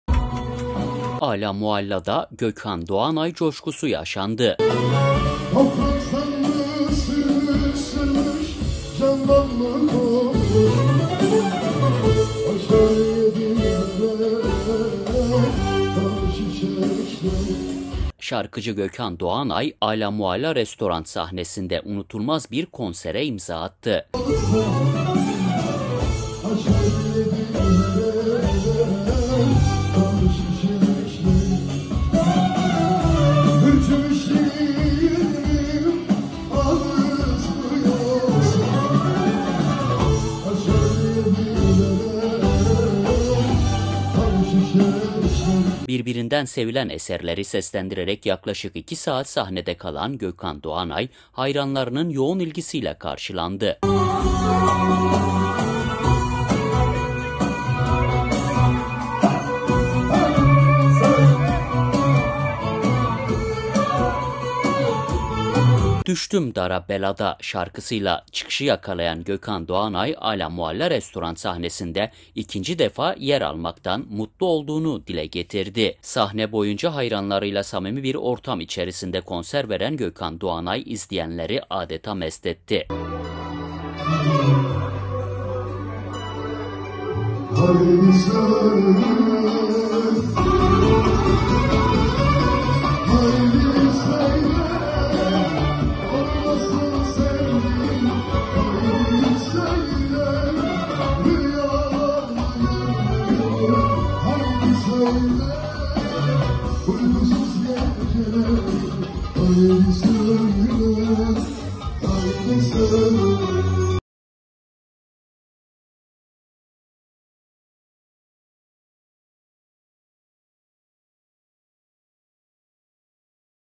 TÜRKÜCÜ GÖKHAN DOĞANAY ALAMUALLADA SAHNE ALDI
Alâ Mualla da Gökhan Doğanay caşkusu yaşandı

Şarkıcı  Gökhan Doğanay Alâ Mualla restaurant sahnesinde unutulmaz bir konsere imza attı.